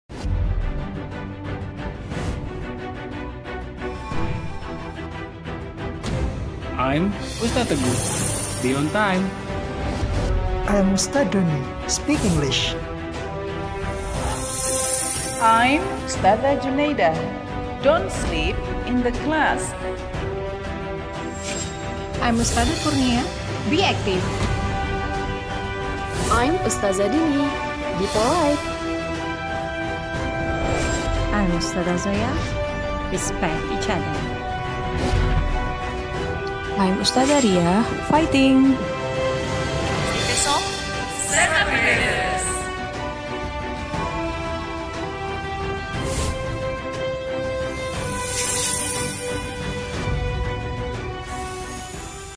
English Team of 7th graders & Class Rules - Mr. D
" Salam Ugluk " 😂😂😂

Hi Teman - Teman, welcome to Mr. D Aditya. Di video ini Mr. D bersama-sama guru2 English Team Smp Progresif Bumi Shalawat kelas 7 menjelaskan tentang peraturan di kelas Bahasa Inggris..
Silahkan kalau ingin request materi B Inggris bisa comment di sini 😄

Kalau teman2 ingin bertanya tentang B Inggris bisa DM saya di facebook dan di Instagram.